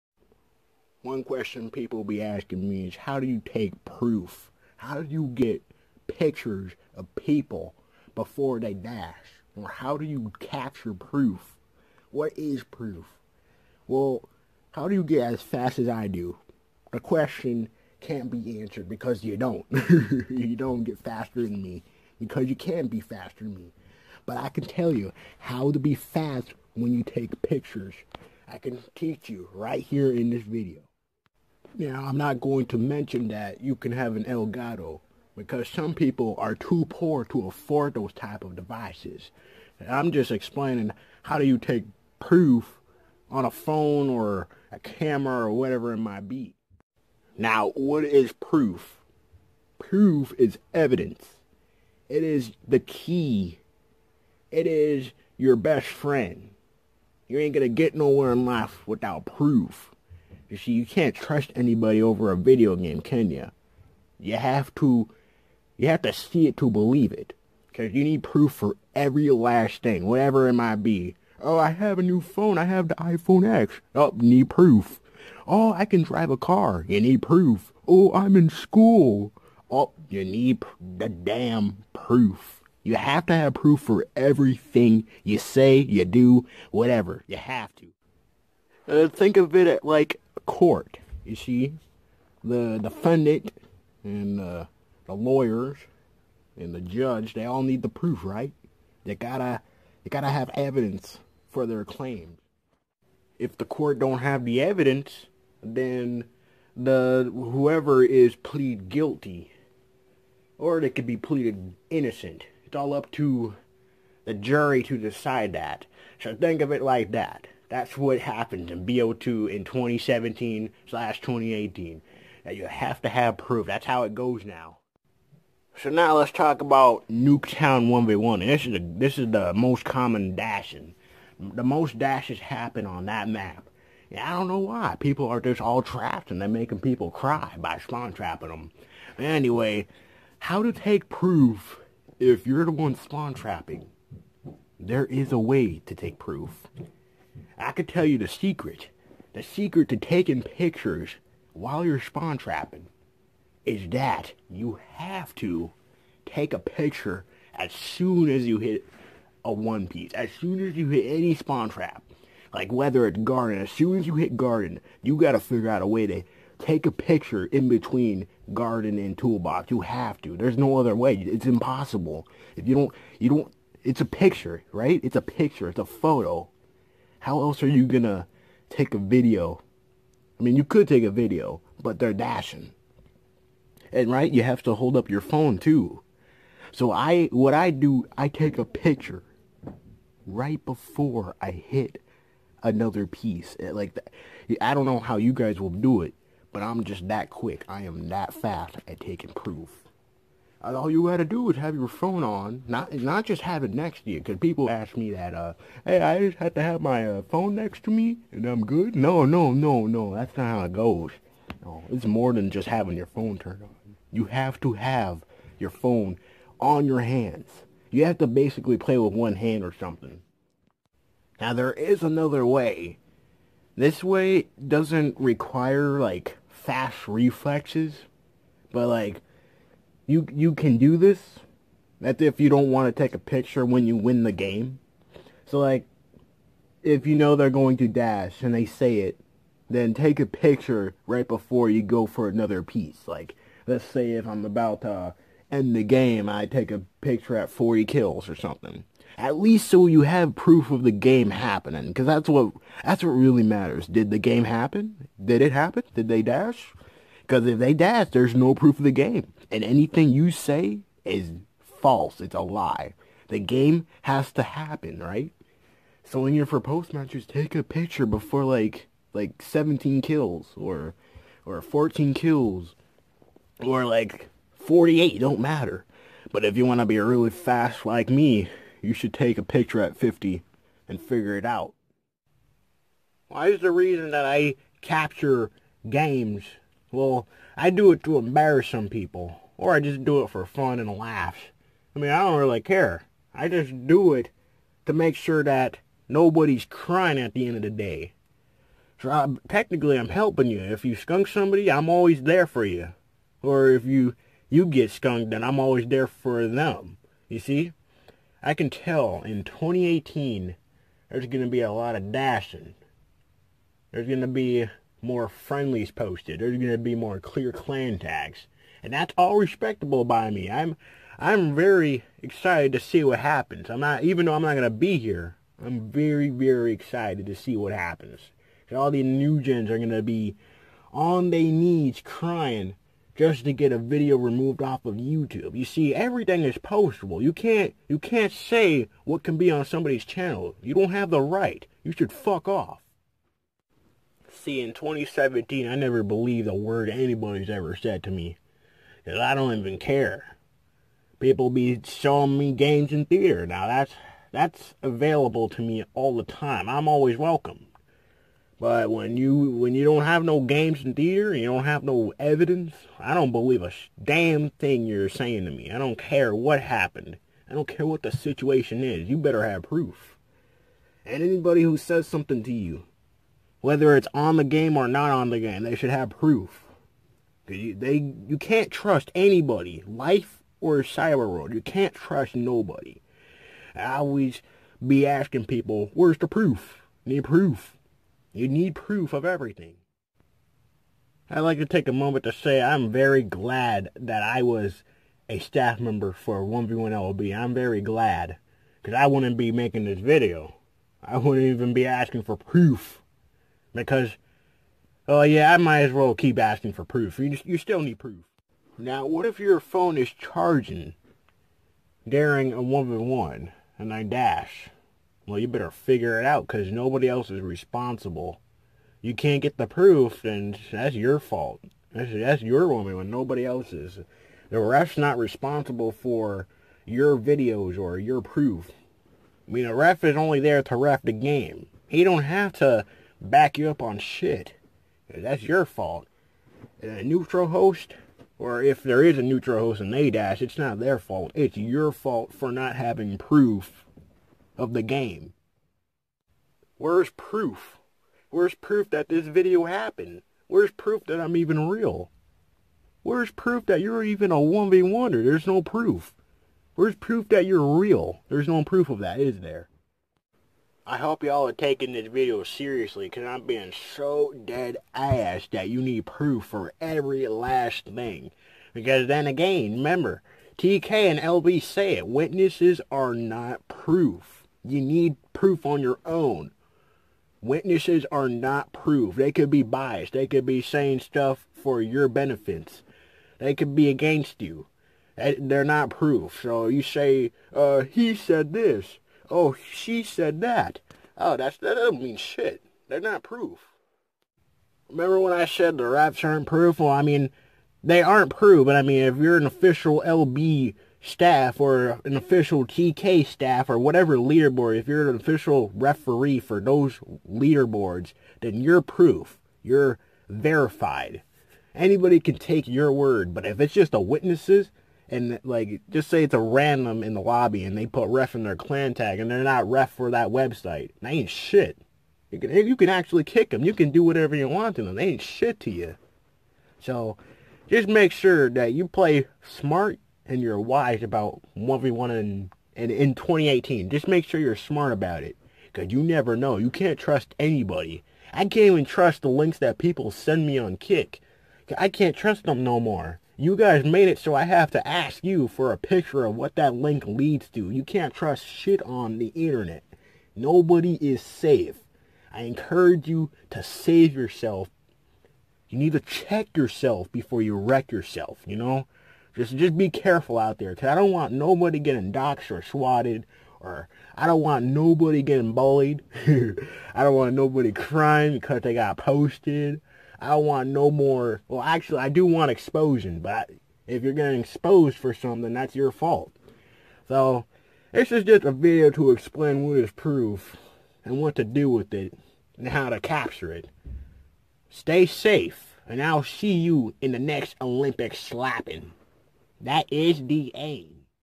Where's The Proof?
I explain what proof really means, why it's important, how to capture it without using a livestreaming device and verification.